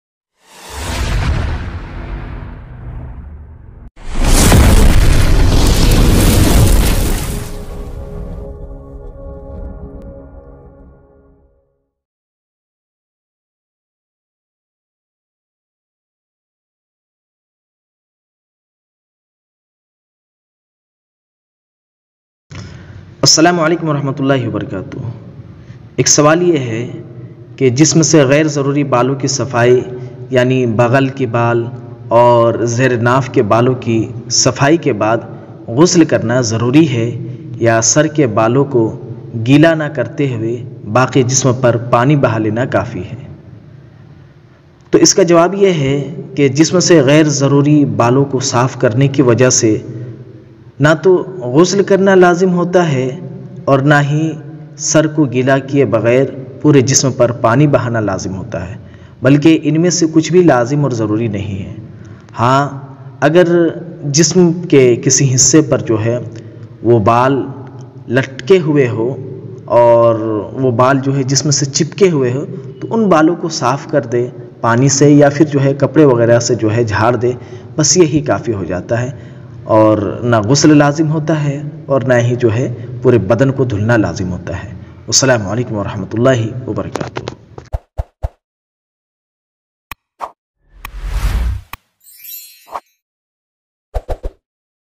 Naf ke neeche aur bagal ke baal saaf karne se gusal karna zaroori hai ? या सर के इलावा पुरा बदन धुले
Is video me aap ko malum hoga ke agar koie naf ke neeche ke baal ya bagal ke baal ko saaf karta hai to gusal karna zaroori hai

Kya bagal or naf ke neeche ke baal saaf karne se pura badan dhona padega sar ke elawa

Ya Kya krna padega

In sare sawalat ka jawab is choti si video me bataya gaya hai

Aap is video ko pura jarur dekhy aur apne duston me bhi sher karen take unhe bhi malum ho